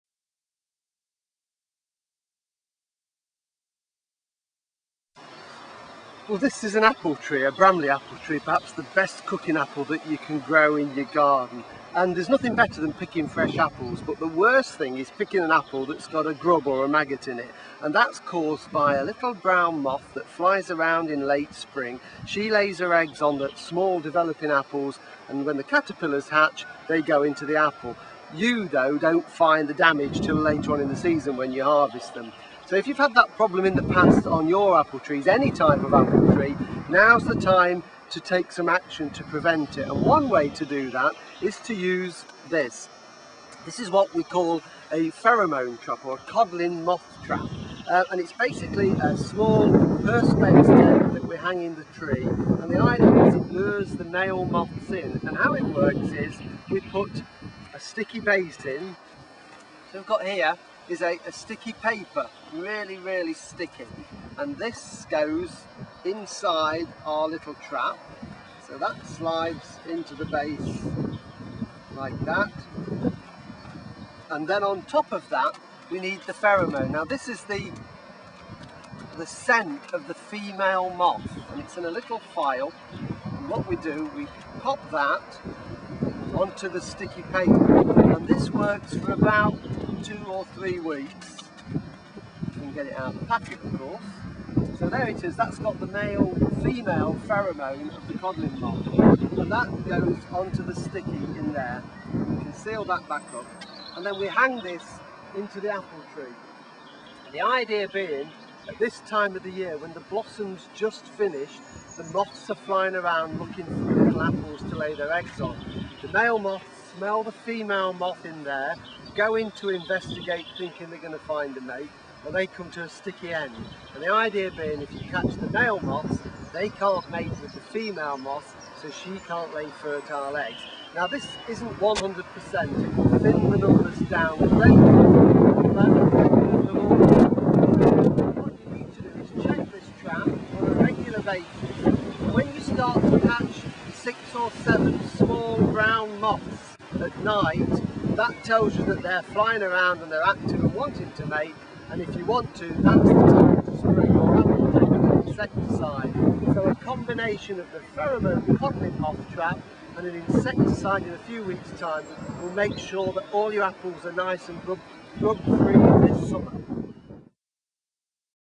Pests In Apple Trees
Codling Moth Control

One of the main pests on apple trees is Codling moth, or the caterpillars of the moth which tunnel into the fruits as they grow.

The damage caused by the burrowing is normally discovered in the summer or when the fruits are picked in autumn, but now is the time to prevent damage this summer.

The moth lays its egg on the fruitlets after blossom drop in late spring and early summer and the small caterpillars start burrowing into the fruits where they cause a great deal of damage.

To reduce the damage caused by the moths you can hang Codling moth traps into your apple trees now.

Inside the triangular trap is a sticky base to which you position a small phial containing the pheromone (hormones of the female moth) to attract the male moths.

When they enter the trap to investigate the scent, they stick to the base of the trap. The idea is if the male moths are trapped, the females can't be fertilised and so the breeding cycle is broken.

Codling moth traps only give partial control because many of the moths will already have mated before they enter the trap in search of more females.

For total control, when you start to catch the small brown moths in the trap it indicates that the moths are active and you can spray you fruit trees with an insecticide to kill the caterpillars as they hatch.